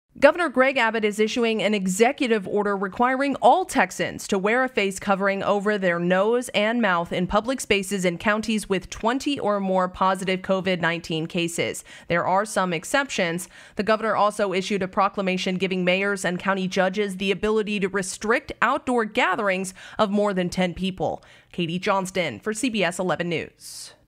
Gov. Greg Abbott Establishes Mask Mandate For Most Texas Counties, Gives Local Power To Restrict Gat
Governor Greg Abbott on Thursday issued an Executive Order requiring all Texans to wear a face covering over the nose and mouth in public spaces in counties with 20 or more positive COVID-19 cases, with few exceptions. Katie Johnston reports.